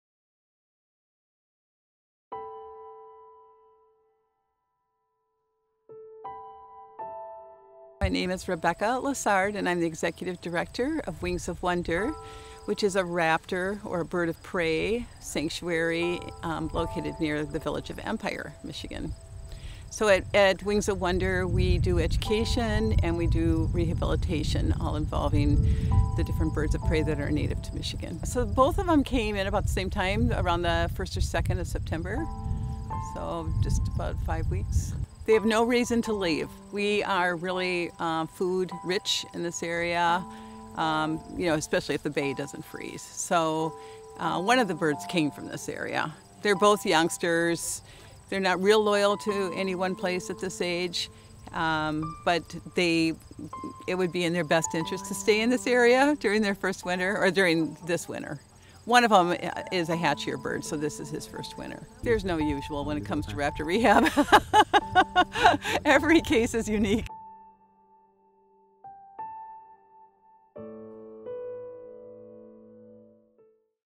GTB/ "Wings of Wonder" Eagle Release
On October 9th, 2019 "Wings of Wonder" a raptor rehabilitation organization, released two adolescent Bald Eagles at the GTB Powwow Grounds in Peshawbestown Mi.